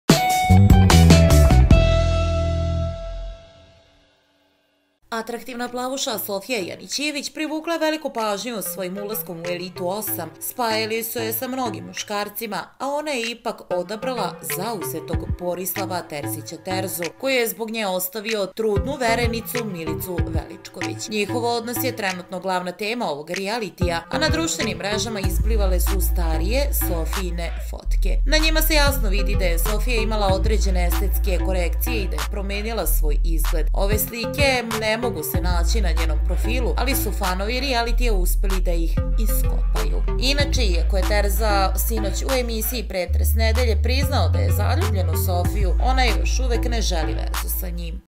OGROMNA RAZLIKA - OVAKO je Sofija IZGLRDALA PRE ESTETSKIH ZAHVATA
Listen to my opinion about Serbian showbiz, Zadruga & Elita latest news - My reaction
Poslušajte nove vesti, zanimljivosti, životne priče, sudbine, Zadrugu, Elitu itd